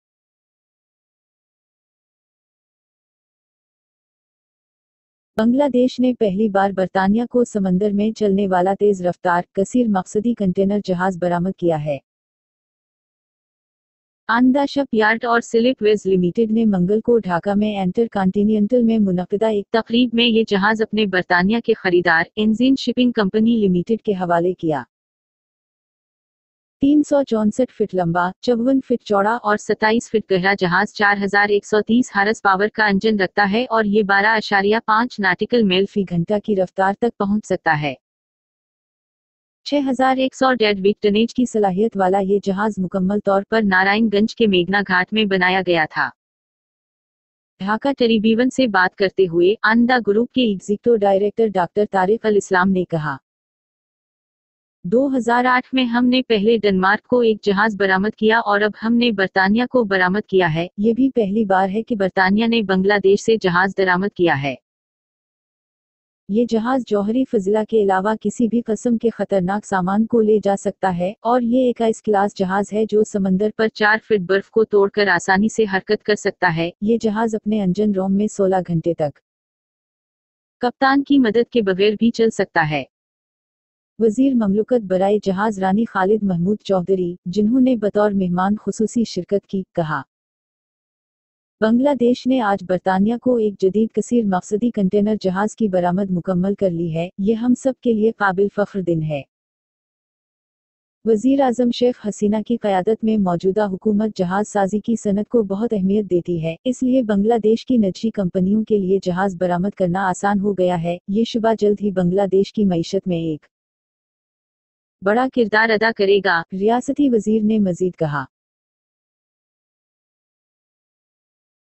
Bangladesh manufactured a cargo ship to England
بنگلہ دیش نے پہلی بار برطانیہ (برطانیہ) کو سمندر میں چلنے والا تیز رفتار، کثیر مقصدی کنٹینر جہاز برآمد کیا ہے۔

 آنندا شپ یارڈ اور سلپ ویز لمیٹڈ نے منگل کو ڈھاکہ میں انٹر کانٹینینٹل میں منعقدہ ایک تقریب میں یہ جہاز اپنے برطانیہ کے خریدار، اینزیئن شپنگ کمپنی لمیٹڈ کے حوالے کیا۔

 364 فٹ لمبا، 54 فٹ چوڑا اور 27 فٹ گہرا جہاز 4,130 ہارس پاور کا انجن رکھتا ہے اور یہ 12.5 ناٹیکل میل فی گھنٹہ کی رفتار تک پہنچ سکتا ہے۔

 6,100 ڈیڈ ویٹ ٹنیج کی صلاحیت والا یہ جہاز مکمل طور پر نارائن گنج کے میگھنا گھاٹ میں بنایا گیا تھا۔

 ڈھاکہ ٹریبیون سے بات کرتے ہوئے، آنندا گروپ کے ایگزیکٹو ڈائریکٹر ڈاکٹر طارق الاسلام نے کہا: "2008 میں ہم نے پہلے ڈنمارک کو ایک جہاز برآمد کیا اور اب ہم نے برطانیہ کو برآمد کیا ہے۔  یہ بھی پہلی بار ہے کہ برطانیہ نے بنگلہ دیش سے جہاز درآمد کیا ہے۔

 "یہ جہاز جوہری فضلہ کے علاوہ کسی بھی قسم کے خطرناک سامان کو لے جا سکتا ہے، اور یہ ایک آئس کلاس جہاز ہے جو سمندر پر چار فٹ برف کو توڑ کر آسانی سے حرکت کر سکتا ہے۔  یہ جہاز اپنے انجن روم میں 16 گھنٹے تک کپتان کی مدد کے بغیر بھی چل سکتا ہے۔

 وزیر مملکت برائے جہاز رانی خالد محمود چوہدری، جنہوں نے بطور مہمان خصوصی شرکت کی، کہا: "بنگلہ دیش نے آج برطانیہ کو ایک جدید کثیر مقصدی کنٹینر جہاز کی برآمد مکمل کر لی ہے۔ یہ ہم سب کے لیے قابل فخر دن ہے۔"

 وزیر اعظم شیخ حسینہ کی قیادت میں موجودہ حکومت جہاز سازی کی صنعت کو بہت اہمیت دیتی ہے۔  اس لیے بنگلہ دیش کی نجی کمپنیوں کے لیے جہاز برآمد کرنا آسان ہو گیا ہے۔  یہ شعبہ جلد ہی بنگلہ دیش کی معیشت میں ایک بڑا کردار ادا کرے گا،” ریاستی وزیر نے مزید کہا۔